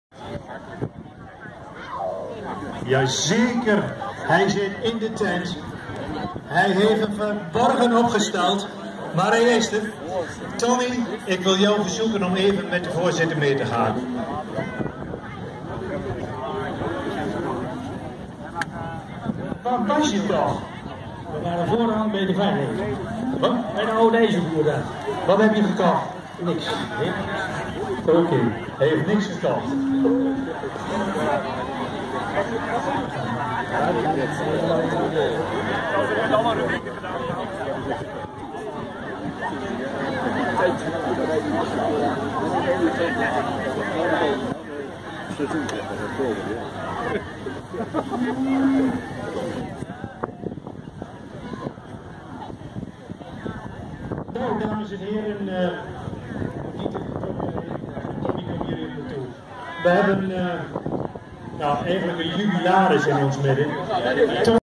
Marie 374 fokveedag Oldebroek aug 2016
Marie 374, de zevende kalfs dochter van Mascol uit een Lord Lily moeder. Inmiddels 68 duizend kg gegeven.
Eigenaar fam. D. Pelleboer, IJsselmuiden